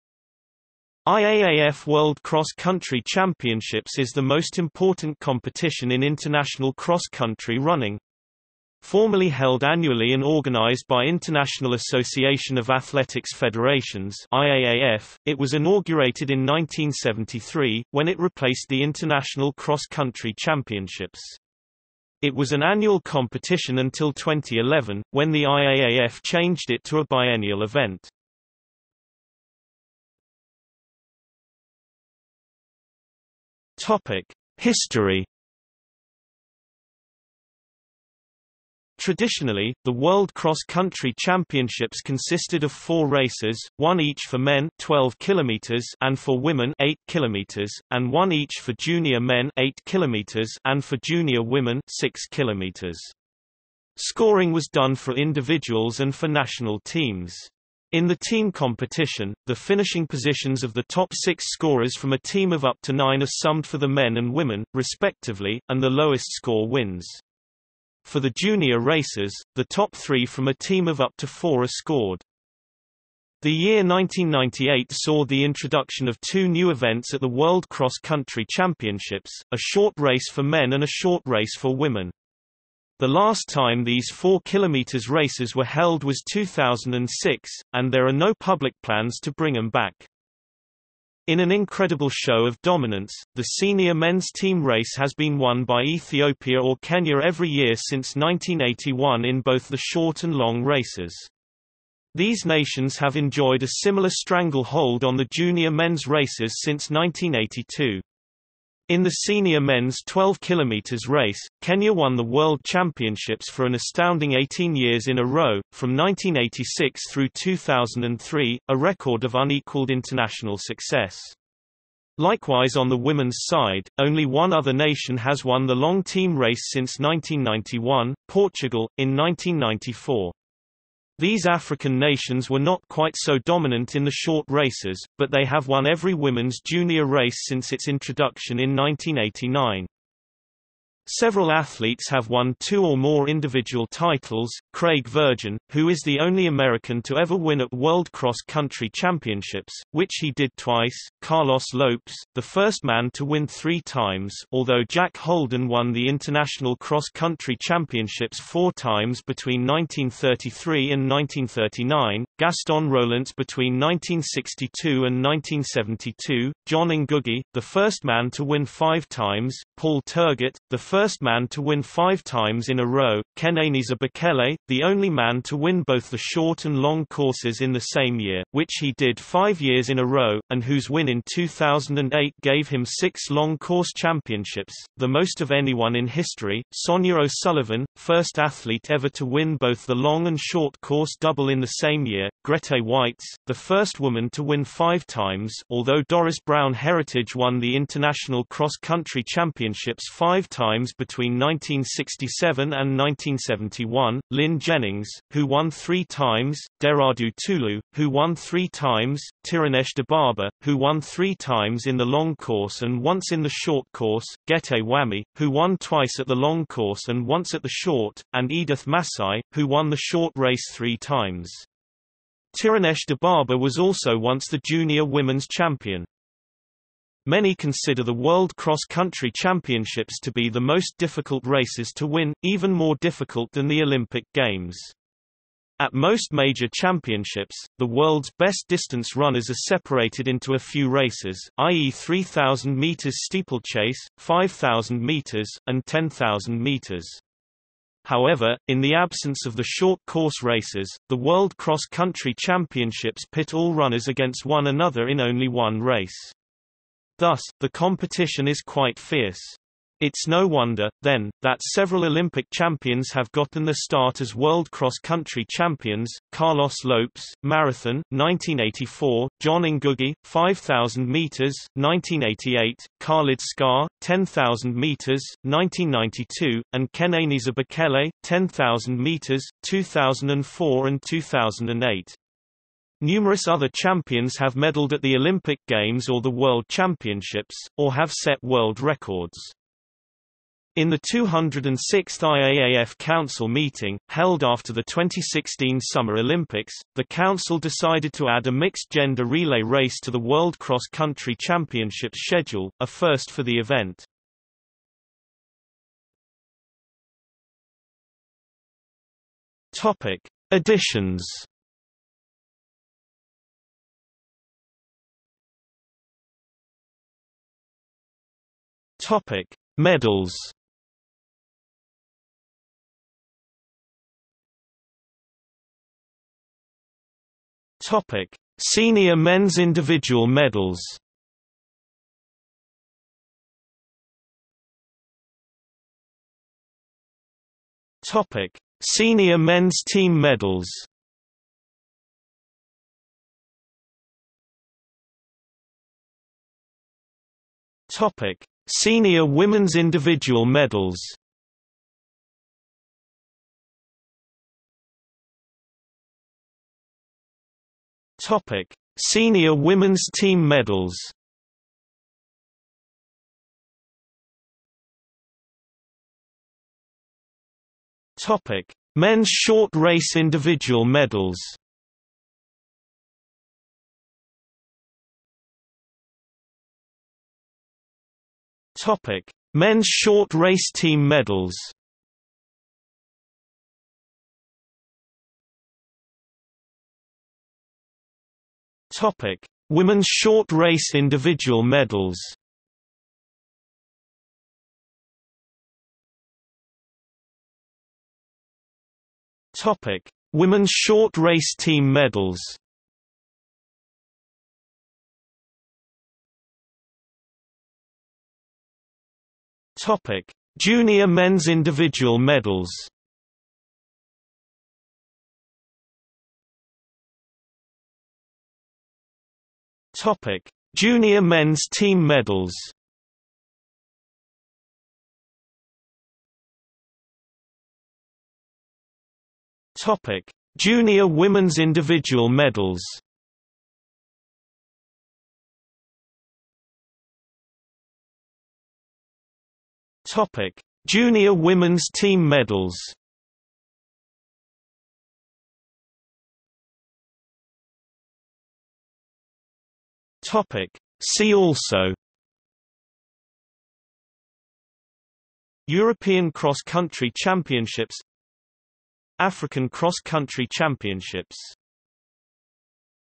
IAAF World Cross Country Championships | Wikipedia audio article
00:00:30 1 History
00:05:06 2 Editions
00:05:15 3 Medals
00:05:24 3.1 Senior Men's Individual Medals
00:05:34 3.2 Senior Men's Team Medals
00:05:44 3.3 Senior Women's Individual Medals
00:05:54 3.4 Senior Women's Team Medals
00:06:04 3.5 Men's Short Race Individual Medals
00:06:15 3.6 Men's Short Race Team Medals
00:06:25 3.7 Women's Short Race Individual Medals
00:06:36 3.8 Women's Short Race Team Medals
00:06:46 3.9 Junior Men's Individual Medals
00:06:56 3.10 Junior Men's Team Medals
00:07:06 3.11 Junior Women's Individual Medals
00:07:16 3.12 Junior Women's Team Medals
00:07:26 4 See also

- Socrates

SUMMARY
IAAF World Cross Country Championships is the most important competition in international cross country running. Formerly held annually and organised by International Association of Athletics Federations (IAAF), it was inaugurated in 1973, when it replaced the International Cross Country Championships. It was an annual competition until 2011, when the IAAF changed it to a biennial event.